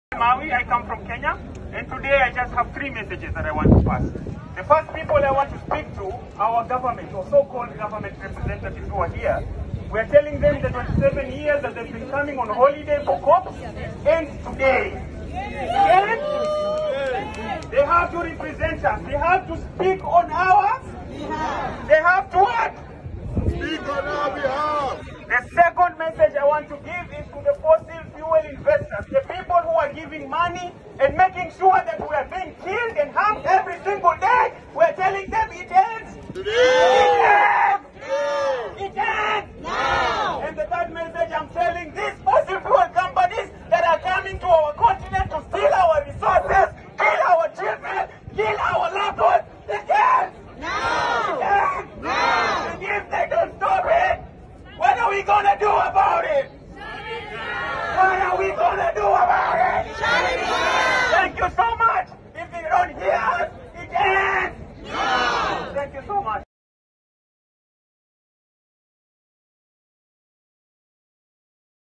Kenyan activist urges African government representatives at COP27 to speak with a strong voice
Omar El Mawi, an activist from Kenya, urging African government representatives at the COP27 to speak with a strong voice.